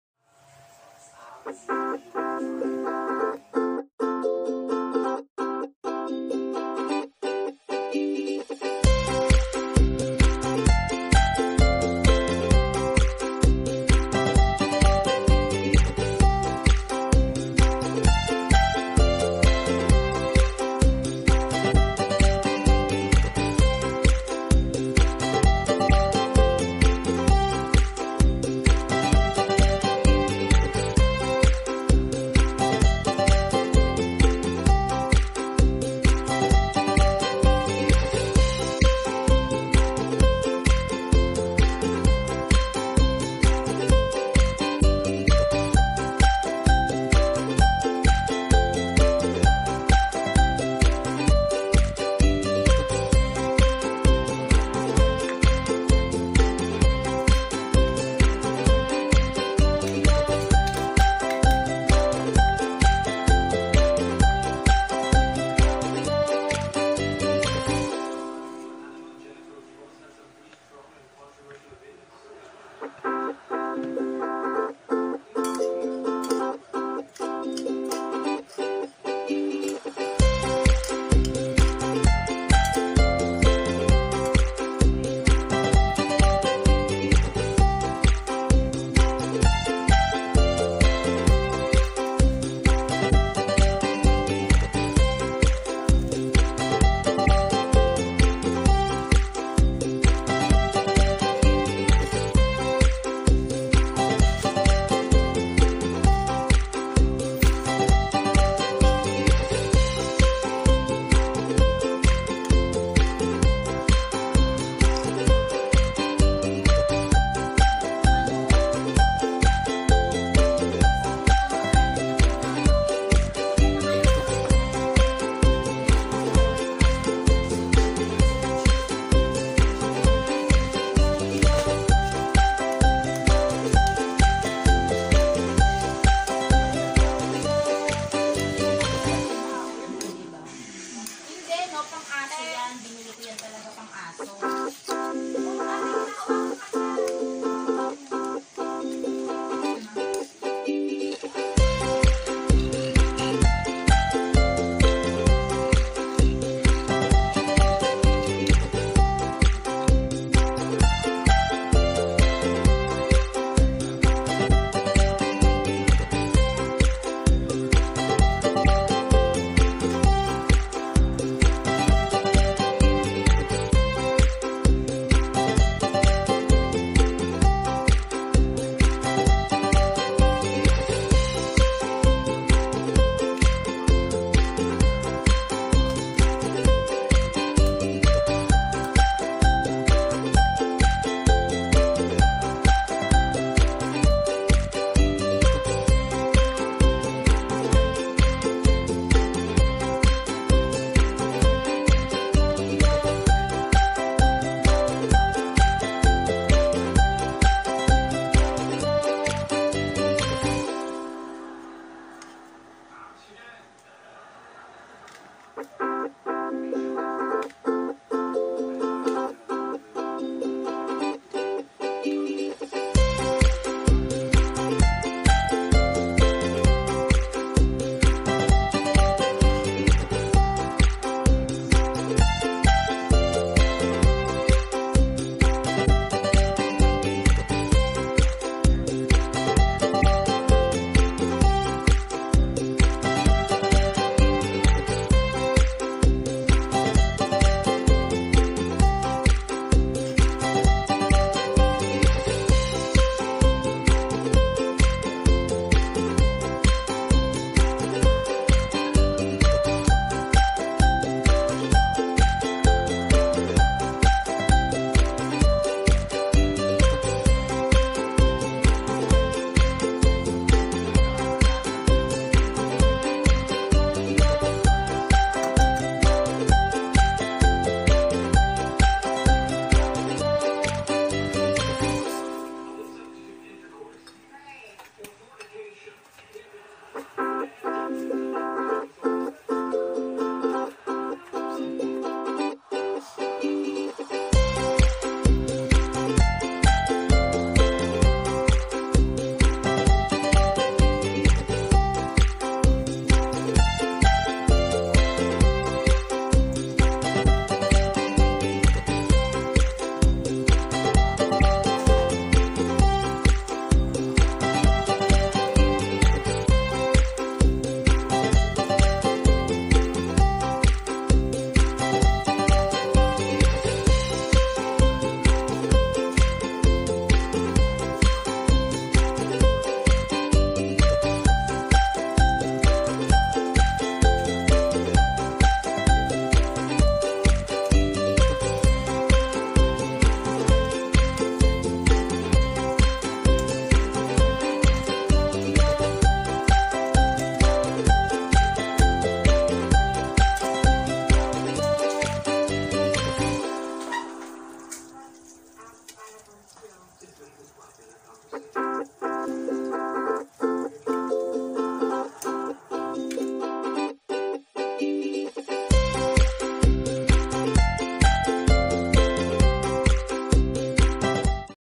Crispy Tofu Budget Friendly ( Pulutan / Merienda / Ulam)
First food vlog

Budget: 40pesos
Ingredients:
4 tofu
2 skyflakes or breadcrumbs
1 cup of flour
1/2 cup of water
Salt and pepper to taste
Paprika optional

Fried it until golden brown